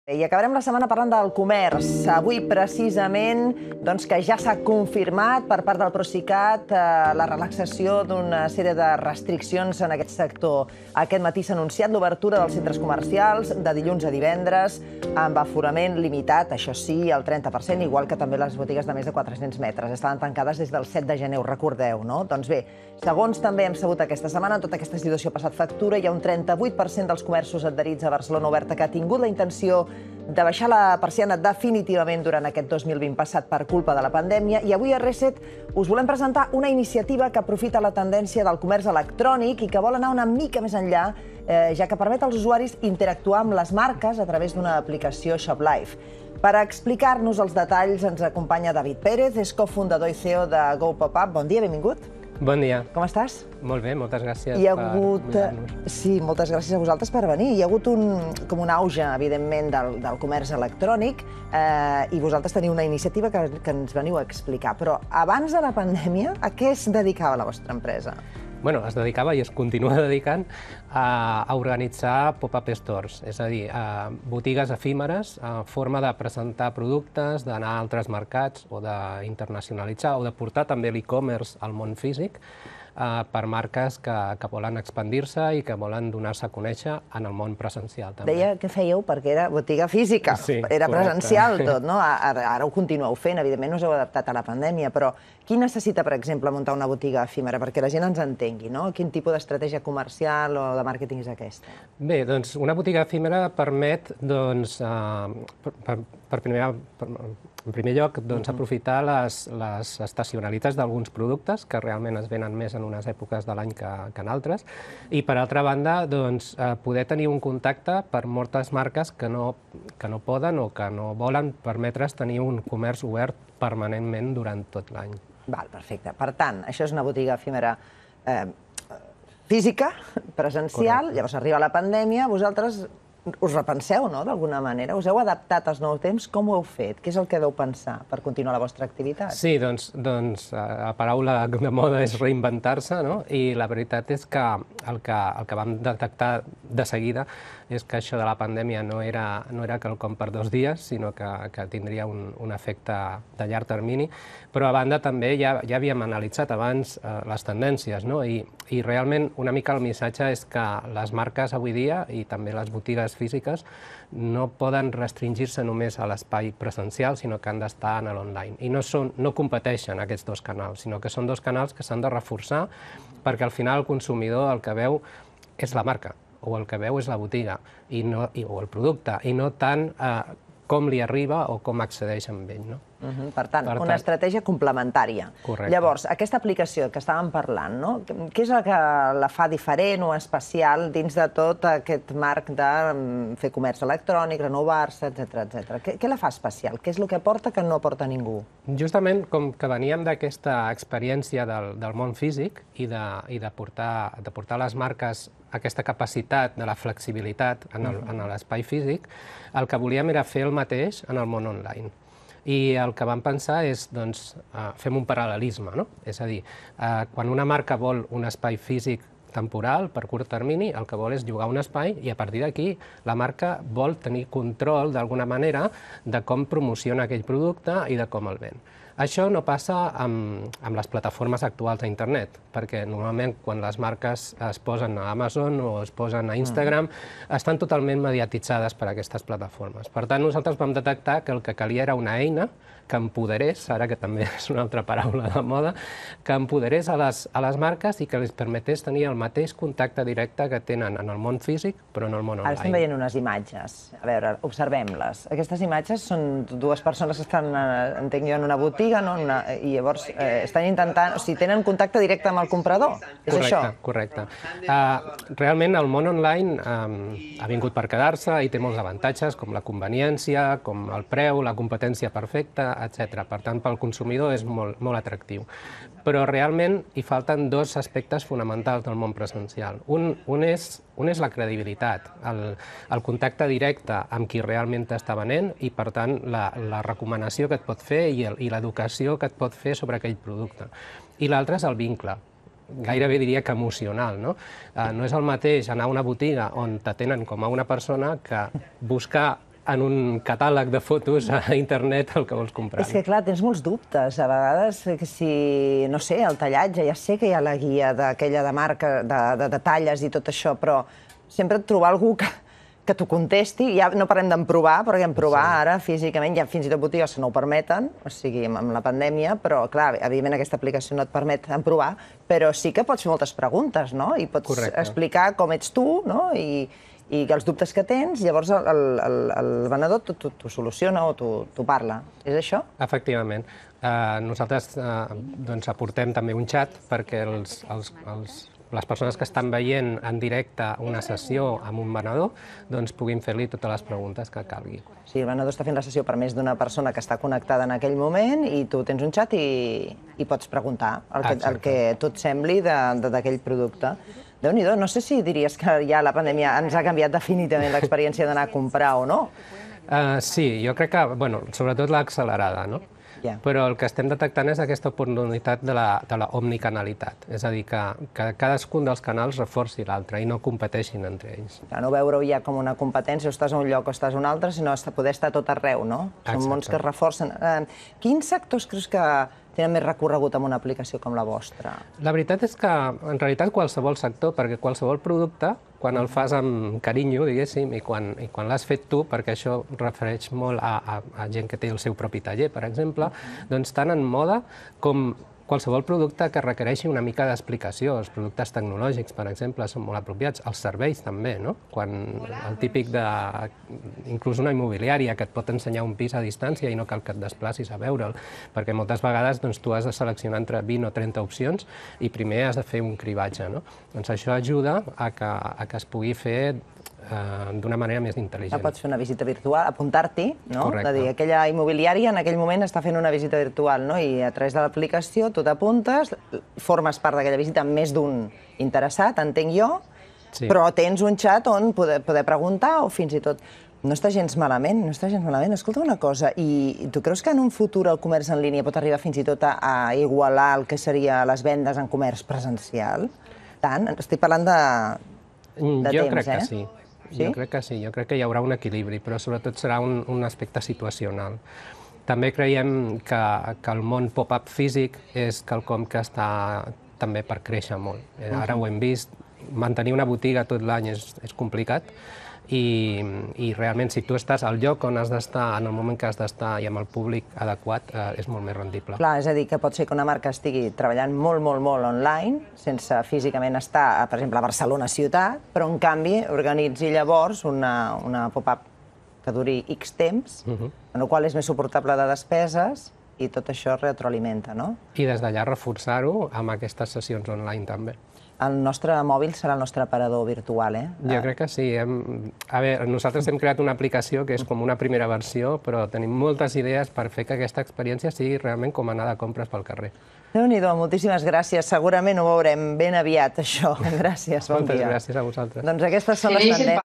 Obteniu més informació sobre Online PopUp
En aquesta breu entrevista amb BETEVE, us mostrarem els avantatges de la nostra eina PopUp en línia que us ajudaran a ser més visibles als vostres clients i a vendre més.